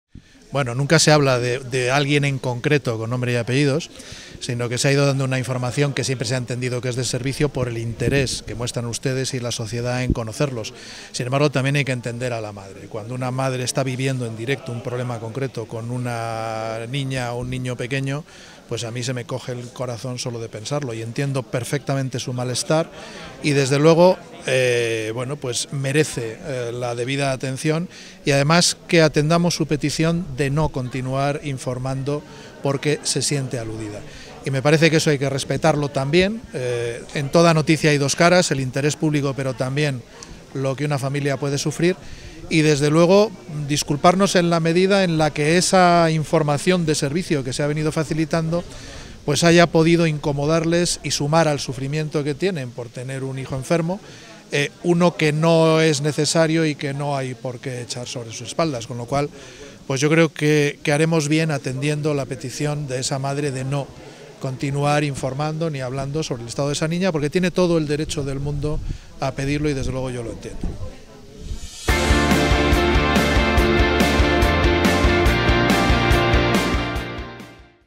López-Escobar pide disculpas: No informará más sobre un caso de meningitis
Javier López-Escobar, delegado territorial de la Junta de Castilla y León en Segovia, ha pedido disculpas este miércoles, tras el malestar manifestado por la familia de una niña enferma de meningitis sobre cuyo estado se ha estado informando “por el interés de la sociedad”: “Nunca se habla de alguien en concreto con nombres y apellidos, sino que se ha ido dando una información que se ha entendido de servicio por el interés de la sociedad. Sin embargo, hay que entender a la madre. Cuando una madre esta viviendo en directo un problema en concreto con un niño pequeño, entiendo perfectamente su malestar y merece la debida atención y que atendamos su petición de no seguir informando porque se siente aludida”, ha dicho.